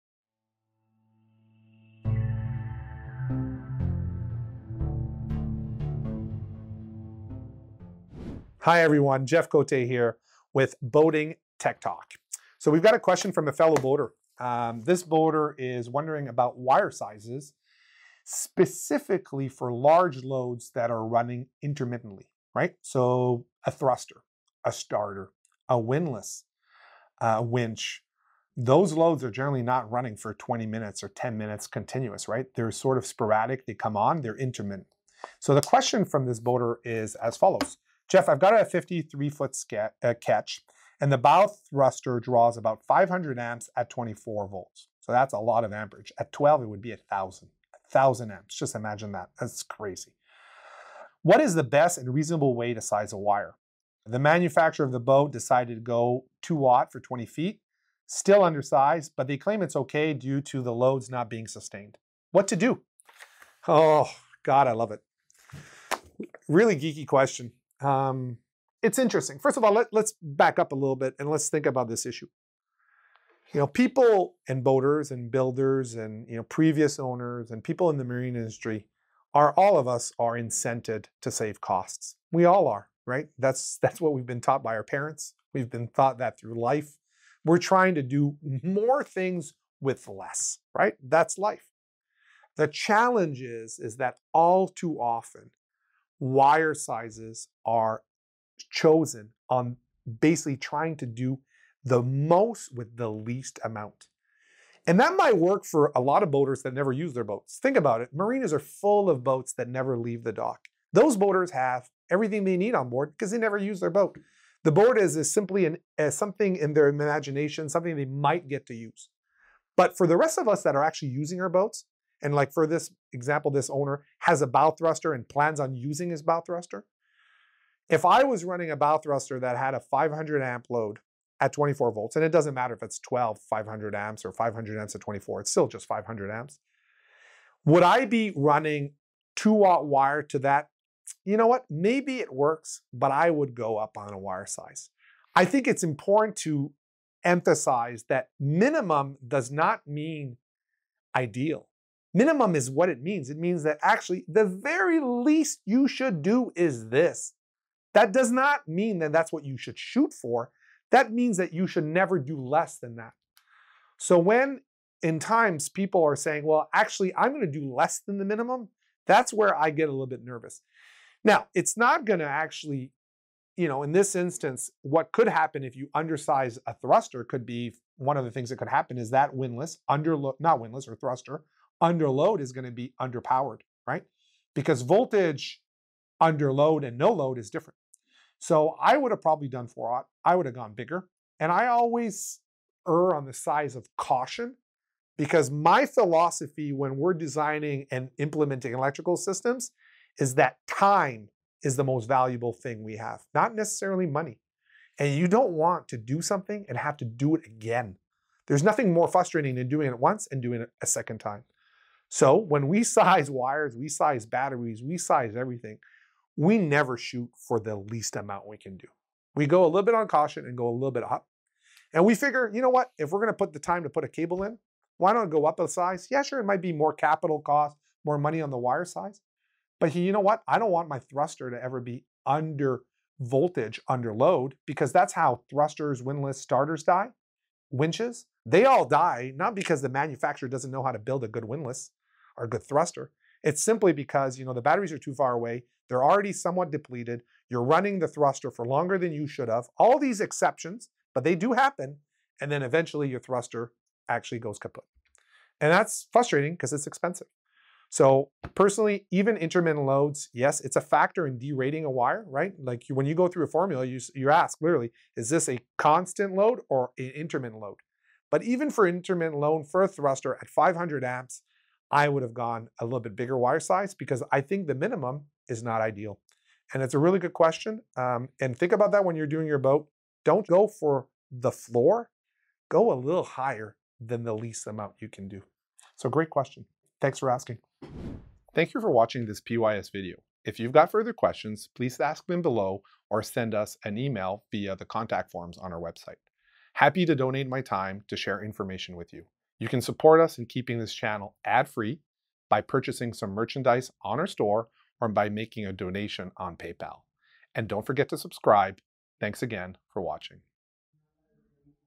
What Is The Best Wire Size for a Bow Thruster?
"Jeff, I love your YouTube series!", writes Terry.  "I have a 53-foot ketch and the bow thruster draws 500A @ 24V for short bursts.  What is the best and reasonable way to size wire?  The manufacturer of the boat chose 2/0 (20 feet) but is still undersized - they claim that is ok because the loads are not sustained.  I am not sure what to do?"

Read our Tech Talk Article from Pacific Yachting Magazine, "The Problems of Summer",

Check out the PYS website for more information on